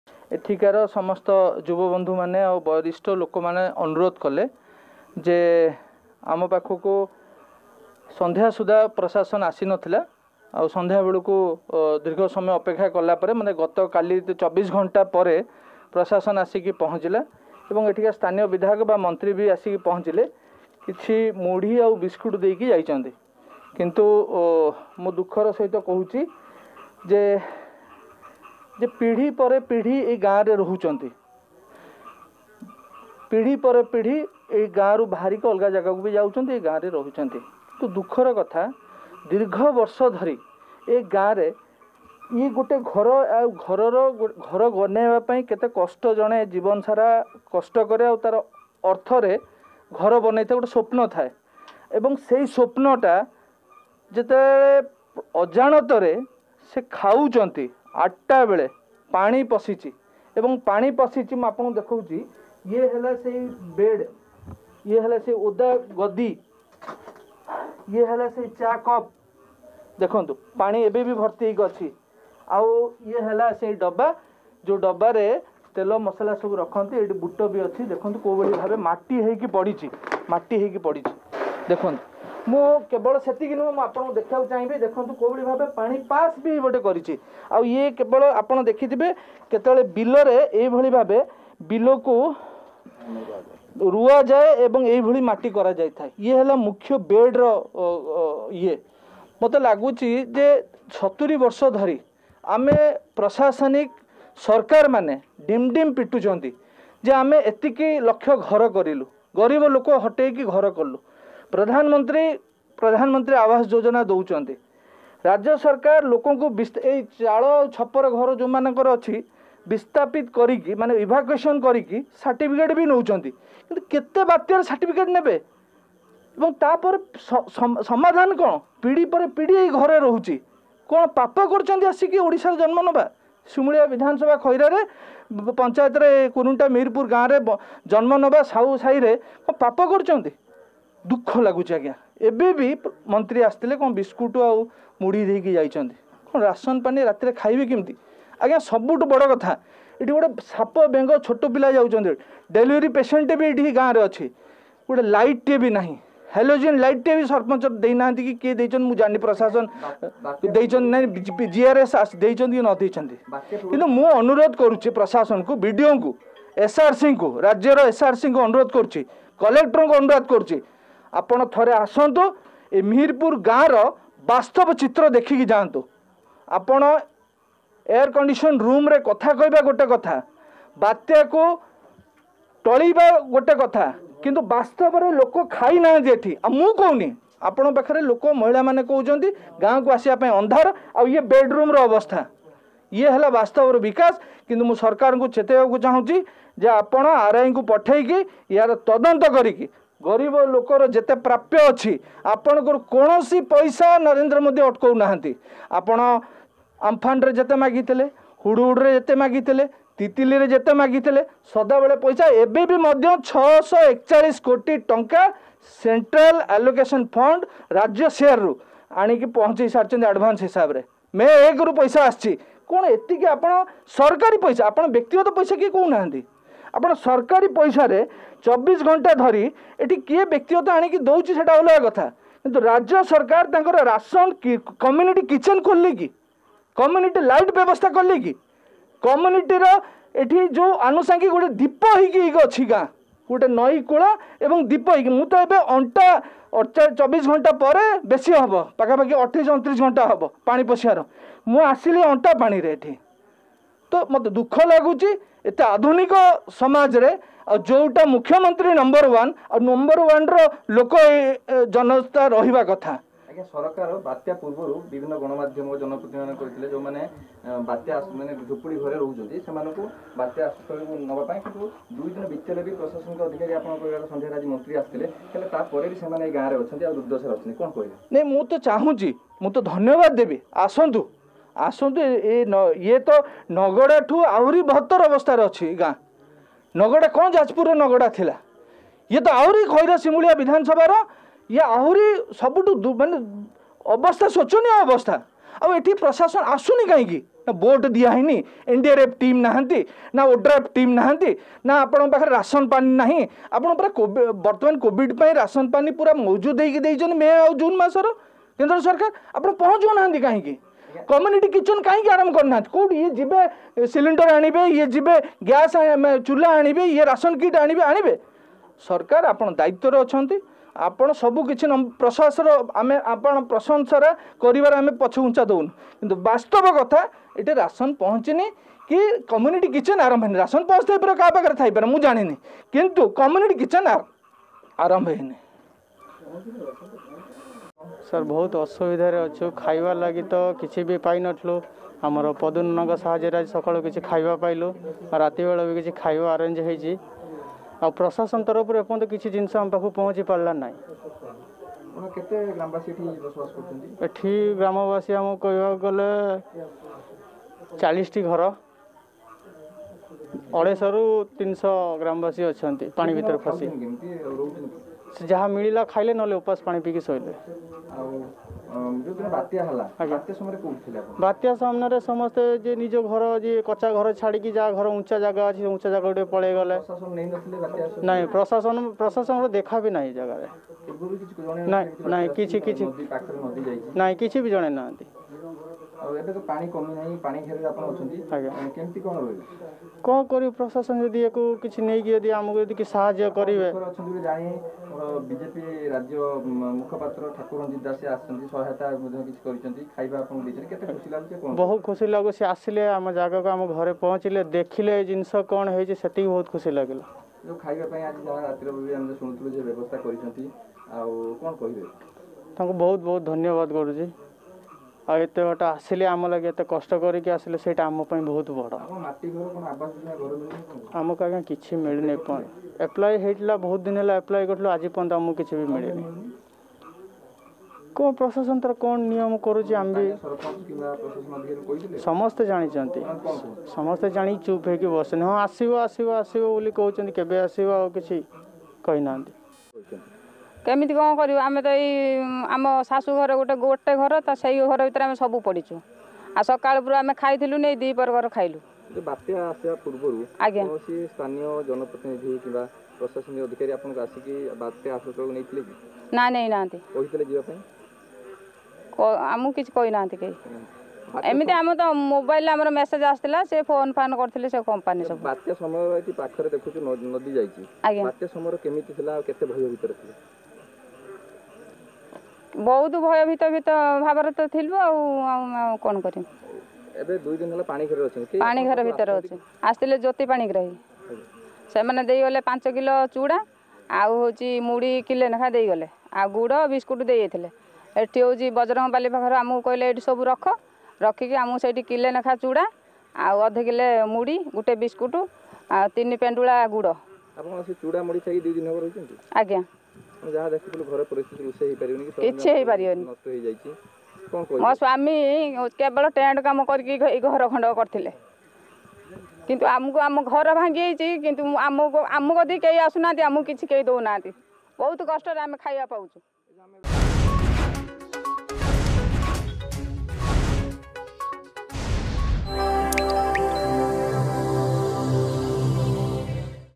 ପାଣି ଘେରରେ ଥିବା ପରିବାରଙ୍କୁ ଭେଟିଲେ ବିଜେପି ନେତା...
ବାଲେଶ୍ୱର ଜିଲ୍ଲା ଖଇରା ବ୍ଲକରେ ବାତ୍ୟା ୟଶ ପ୍ରଭାବରେ ଅନେକ କ୍ଷୟକ୍ଷତି ଘଟିଛି । ବାତ୍ୟାକୁ ଦୁଇଦିନ ବିତିଯାଇଥିଲେ ମଧ୍ୟ ଅନେକ ଗ୍ରାମ ଏବେବି ପାଣି ଘେରରେ ରହିଛି । ଖବର ପାଇ ବିଜେପି ରାଜ୍ୟ ମୁଖପାତ୍ର ଠାକୁର ରଞ୍ଜିତ ଦାସ ଏବଂ ତାଙ୍କର କର୍ମୀମାନେ ପରିବାରଙ୍କୁ ଭେଟି ରନ୍ଧା ଖାଦ୍ୟ ସାମଗ୍ରୀ ଦେବାସହିତ ପରିବାରଙ୍କୁ ଆଶ୍ୱସ୍ତି ଦେଇଥିଲେ...